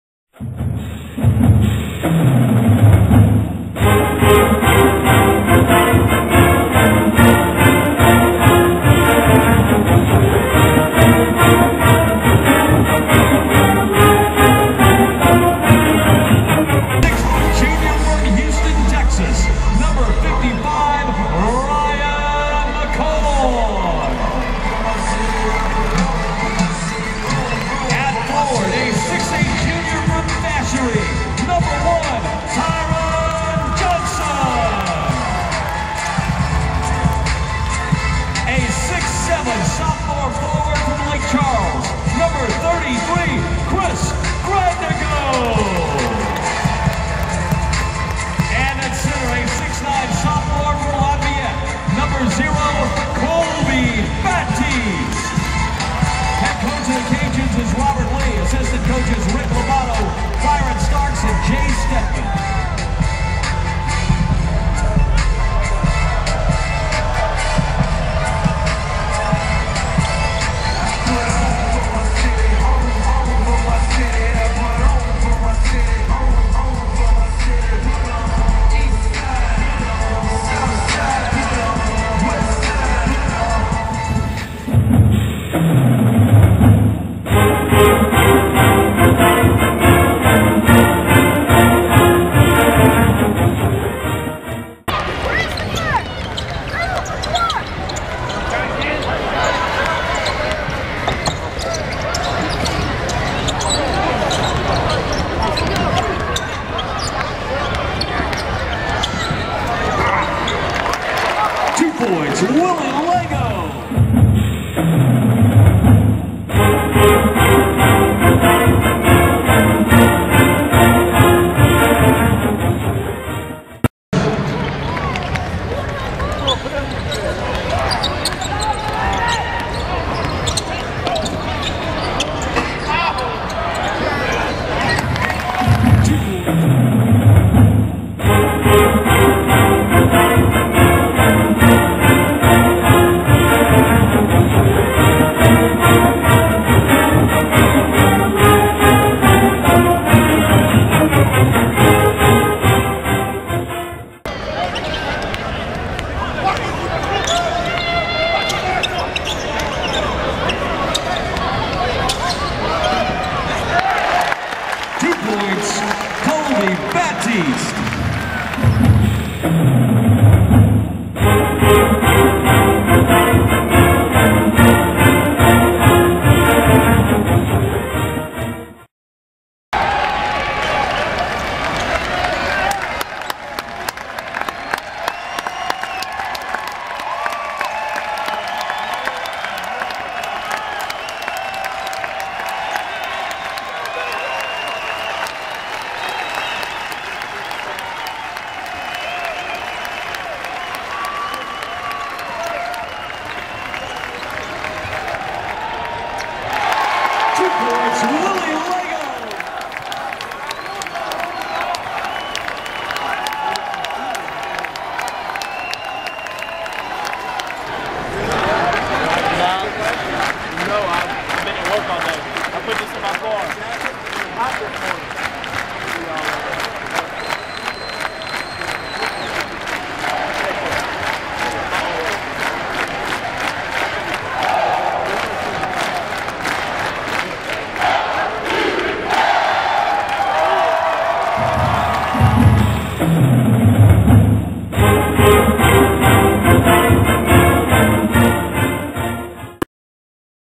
University of Louisiana Ragin Cajun Basketball
Clips and pics from early in the 2008 season (December) vs Louisiana College and UL-Monroe.